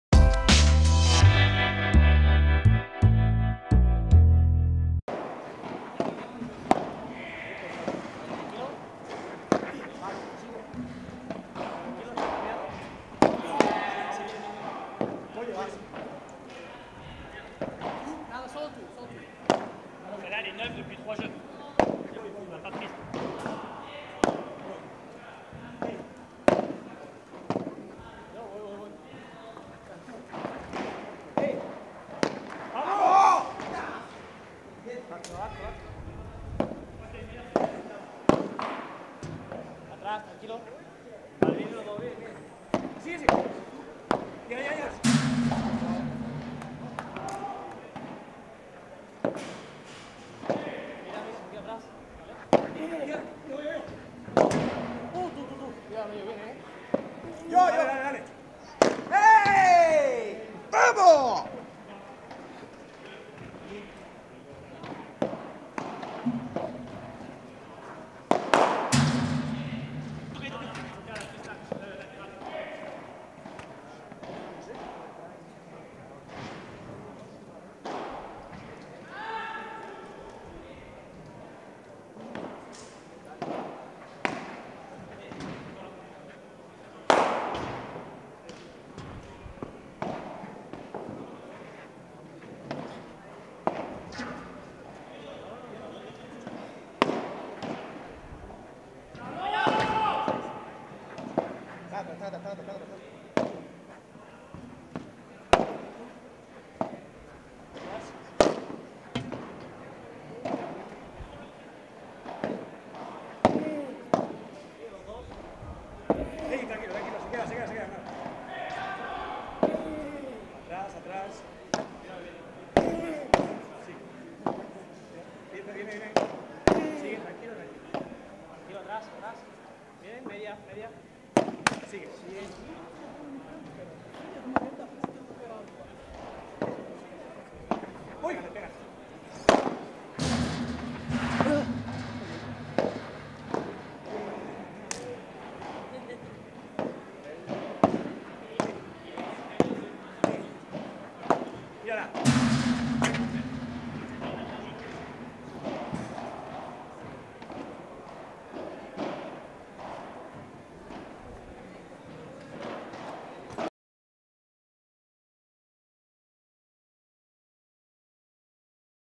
Vamos Pádel. David Leal y Rubén Sánchez contra Fran Jurado y Vasco Pascual. WPT Mijas 2019
Resumen del partido entre David Leal y Rubén Sánchez contra Fran Jurado y Vasco Pascual en las pre previas del WPT Mijas 2019 en el Cerrado del Águila
Resultado: 6/3 6/4